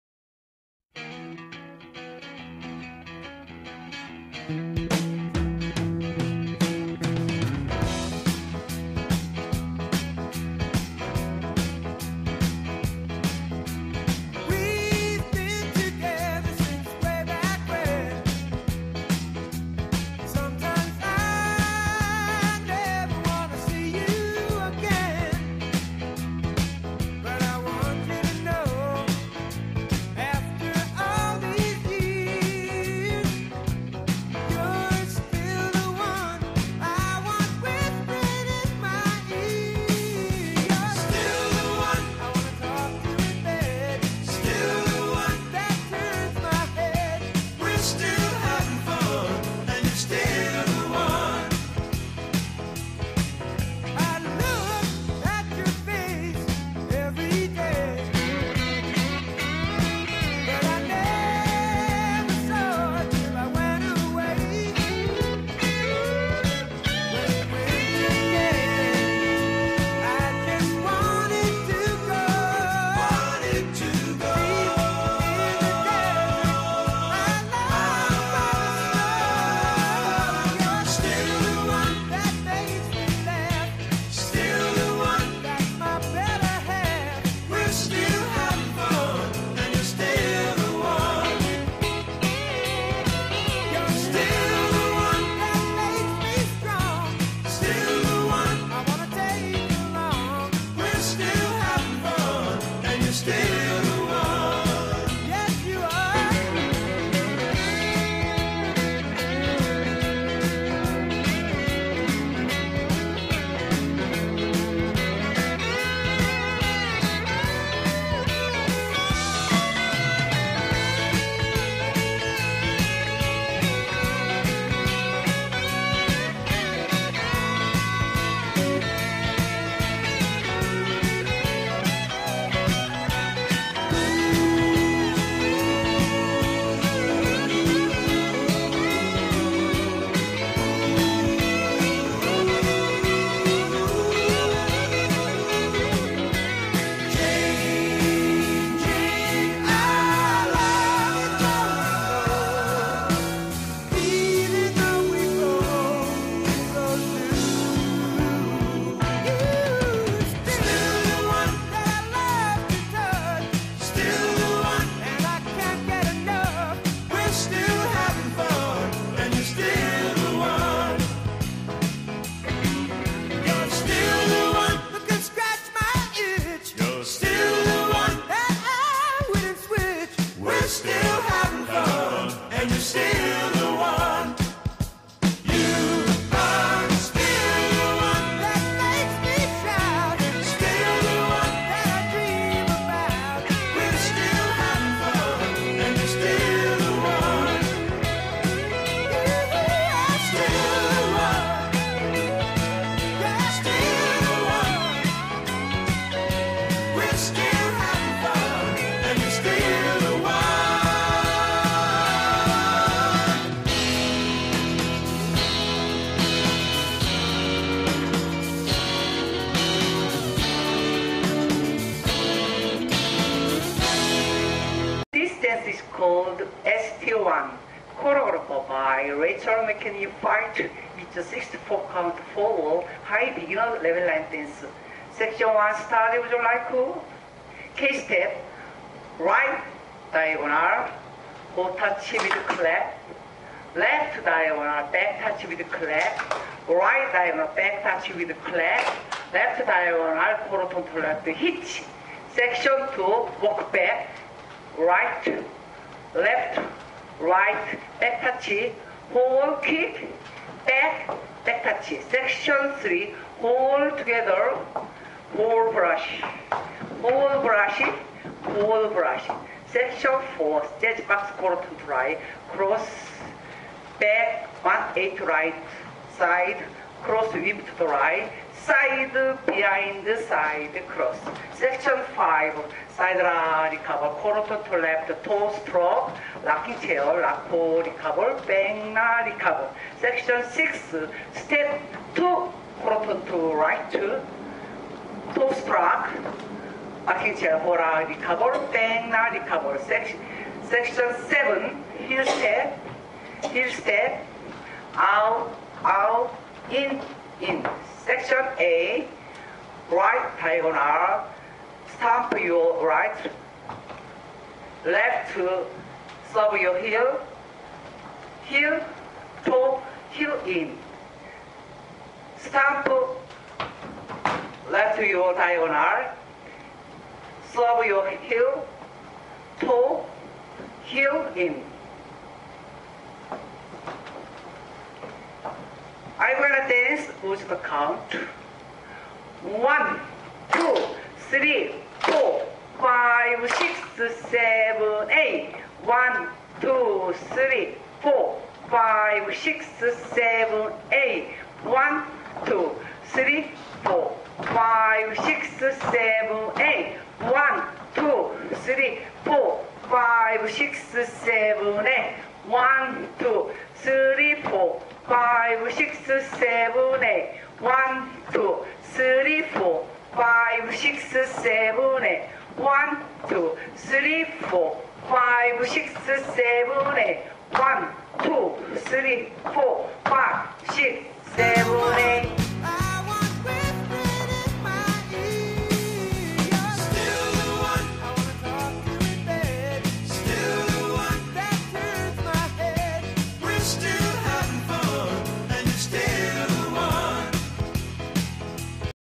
S.T.One (Demo & Walk- Through)(High Beginner)
Line Dance : S.T. One
Choreographed : by Rachael McEnaney ( UK/USA) Mar2016
64C, 4W, High Beginner Level Line Dance
Danced by You Young Hae & Happyclass